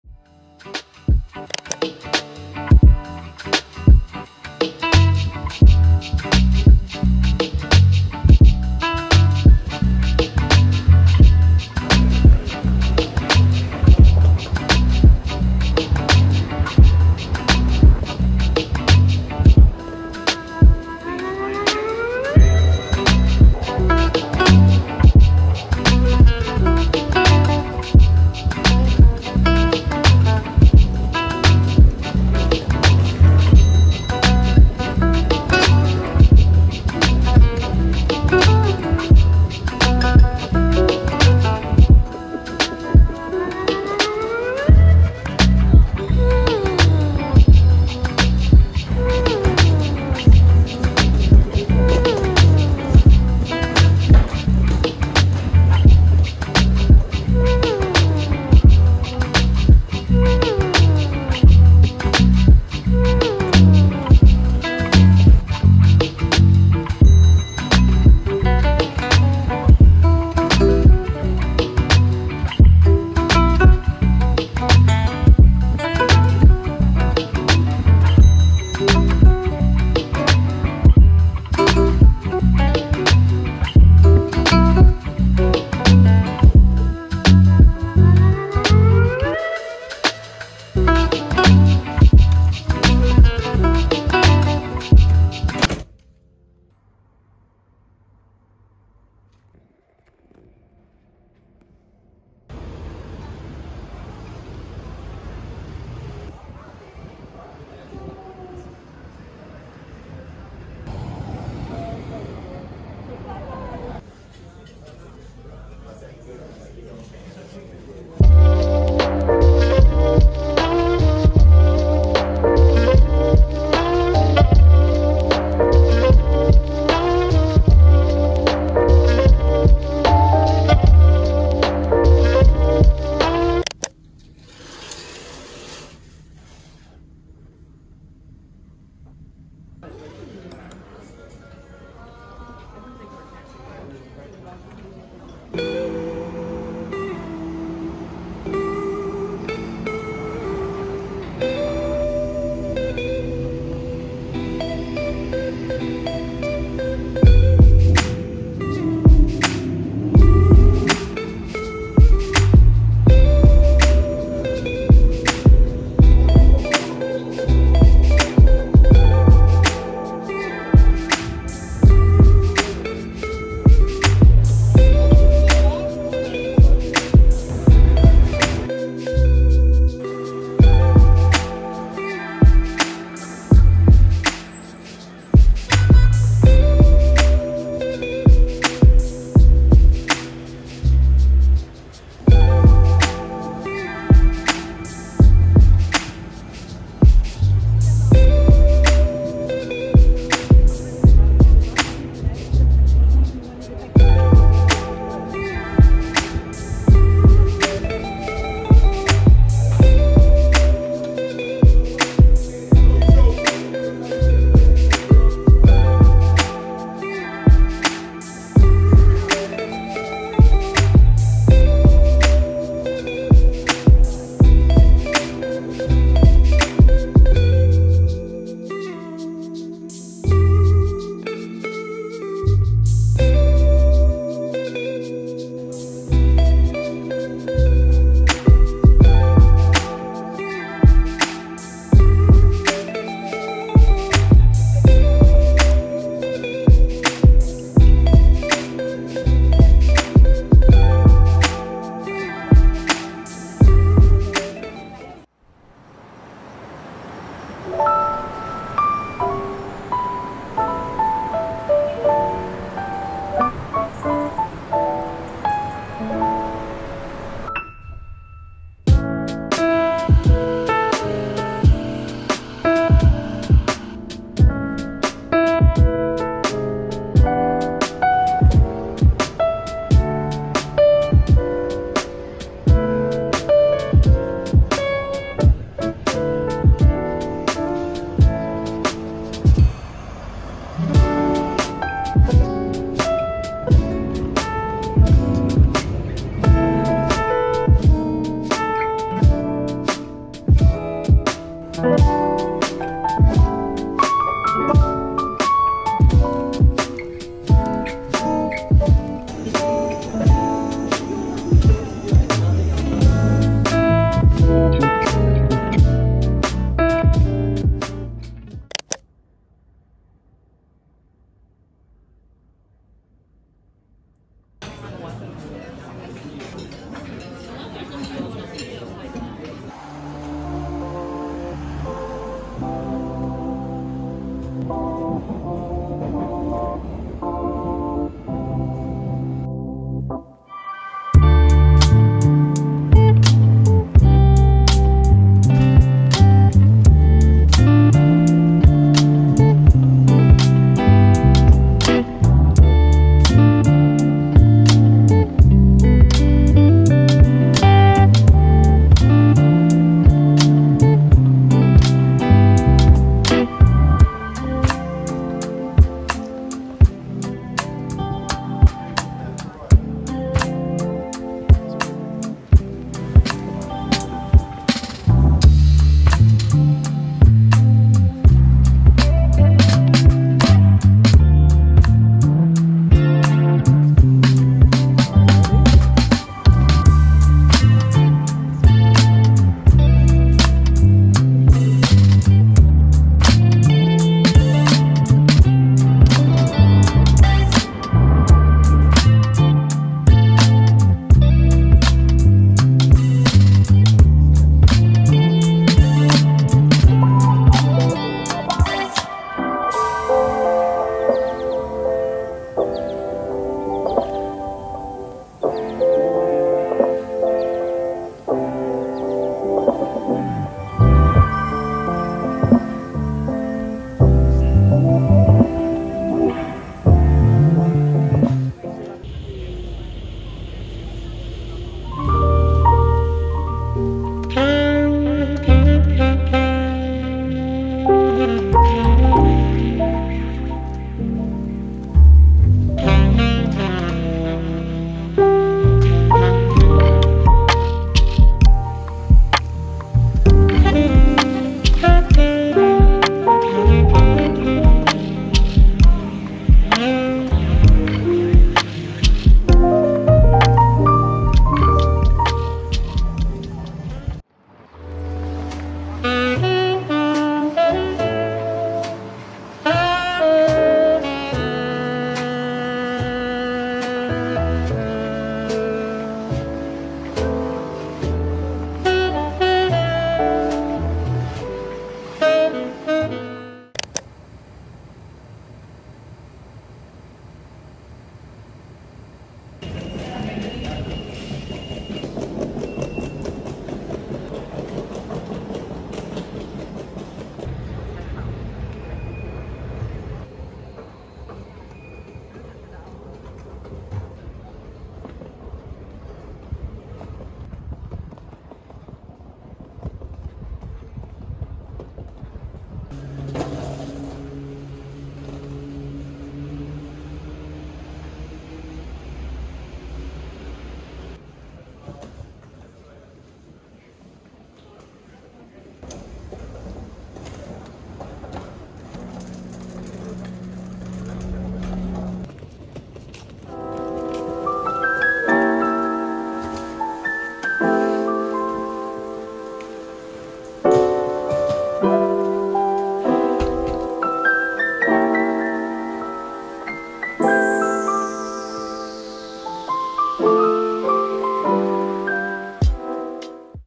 A Weekend in London | Chill Vlog 03
Went to London over the weekend. Also visited Windsor, Stonehenge and Oxford. Happy Sunday!